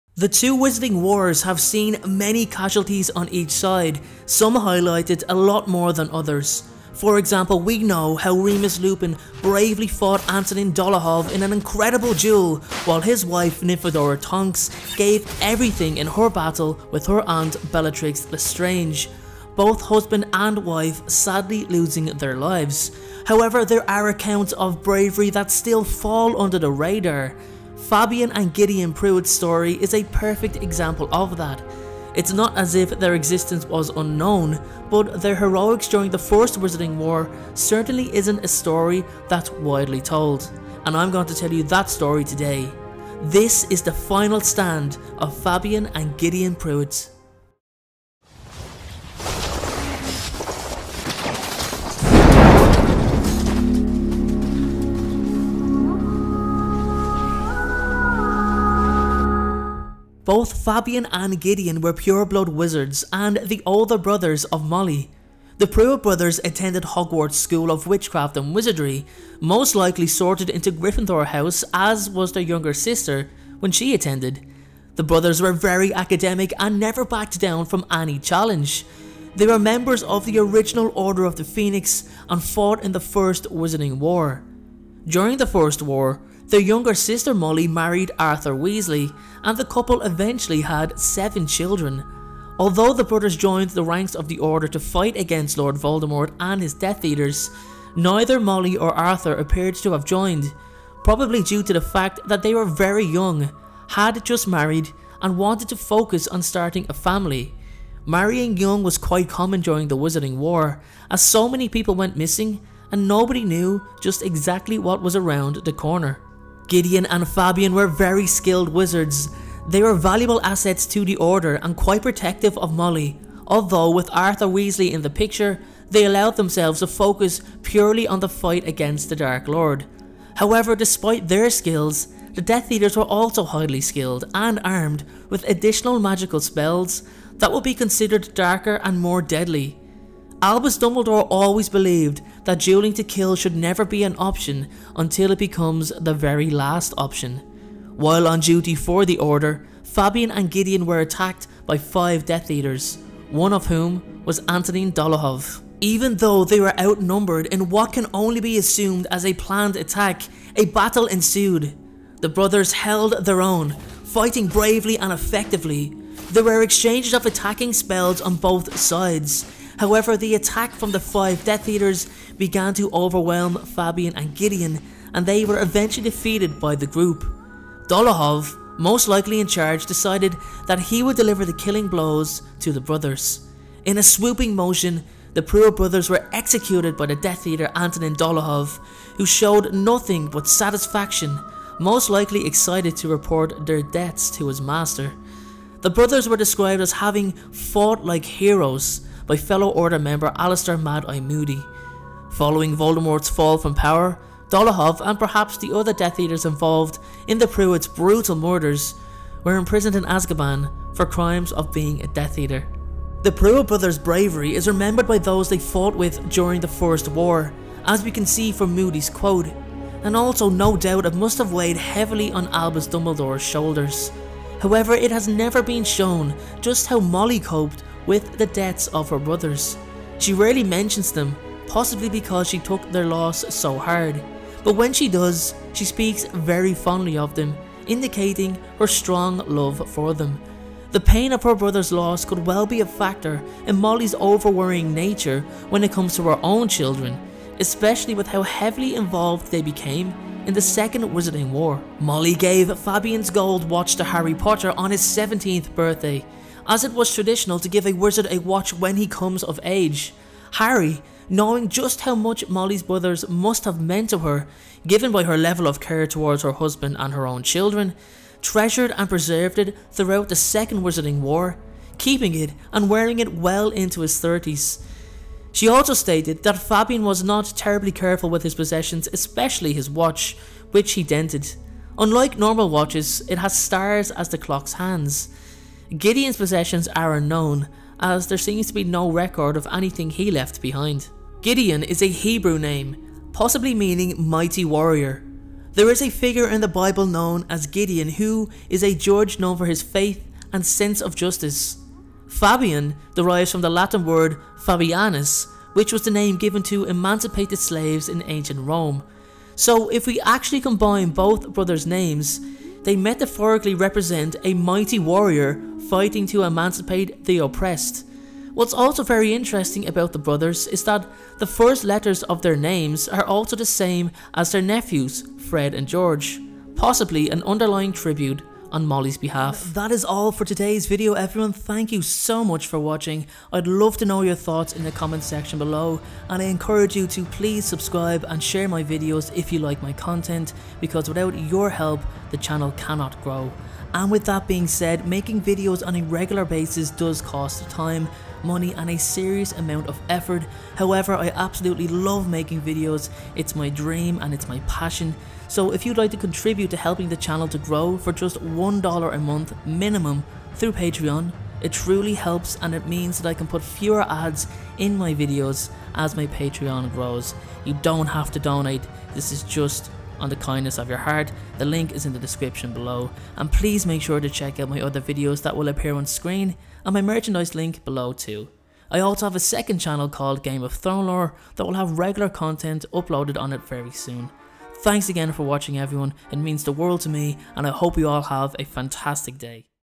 Forgotten Heroes: The Final Stand Of Fabian & Gideon Prewett
Throughout the many years of fighting between the Order and the Death Eaters, we have seen many casualties on each side, some highlighted a lot more than others..we know how Remus Lupin bravely fought Antonin Dolohov in an incredible duel while his wife Nymphadora Tonks gave everything in her battle with bellatrix lestrange…both husband and wife sadly losing their lives..However there are accounts of bravery that still fall under the radar. Fabian and Gideon Prewett’s story is a perfect example of that..It’s not as if their existence was unknown but their heroics for the order certainly isn’t a story that’s widely told.. This is their story..

Forgotten Heroes: The Final Stand Of Fabian and Gideon Prewett.